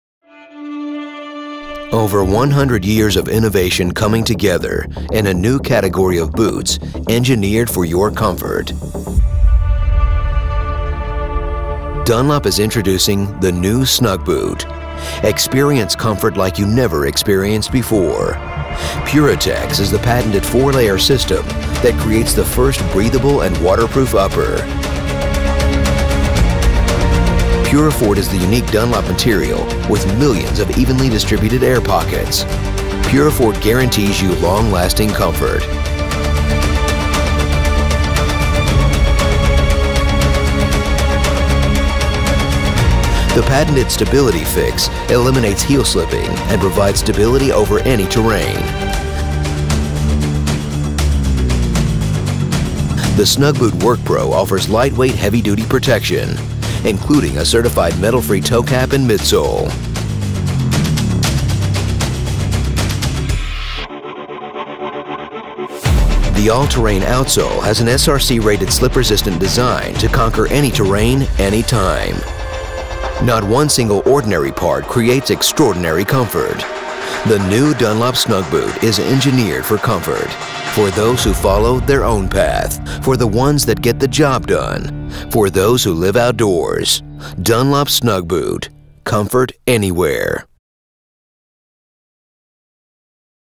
Dunlop is introducing the new Snugboot: Comfort Anywhere
Over 100 years of innovation coming together in a new category of boots engineered for your comfort.

Dunlop is introducing the new Snugboot. Experience comfort like you never experienced before